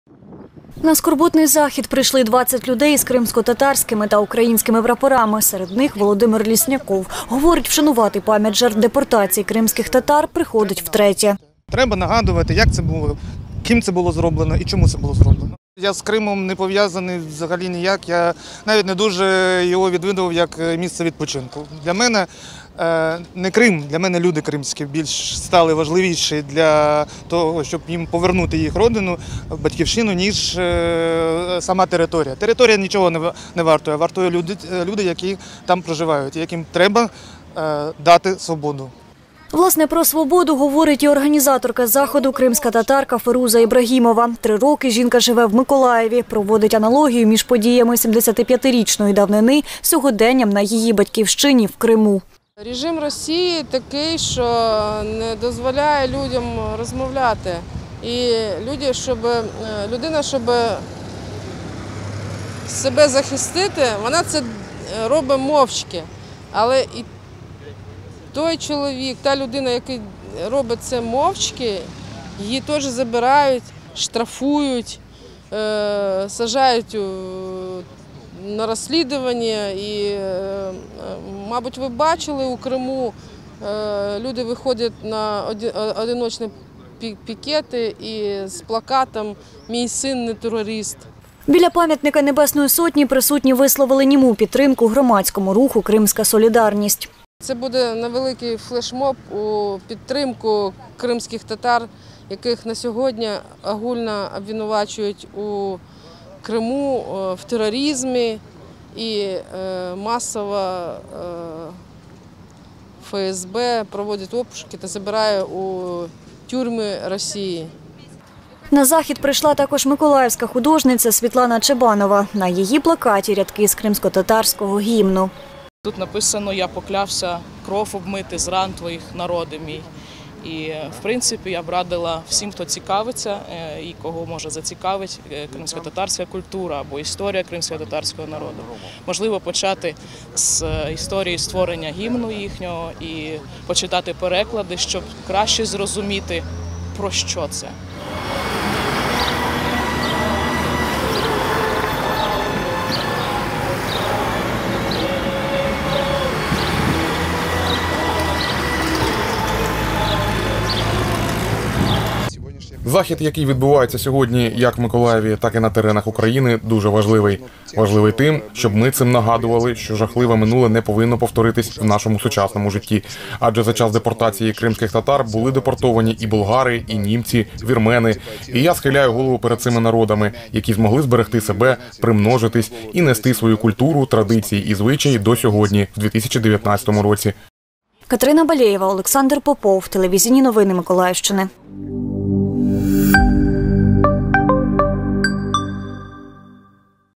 У Миколаєві вшановували пам'ять жертв депортації кримських татар
У Миколаєві, біля пам’ятника Небесній сотні, вшановували пам'ять жертв депортації кримських татар.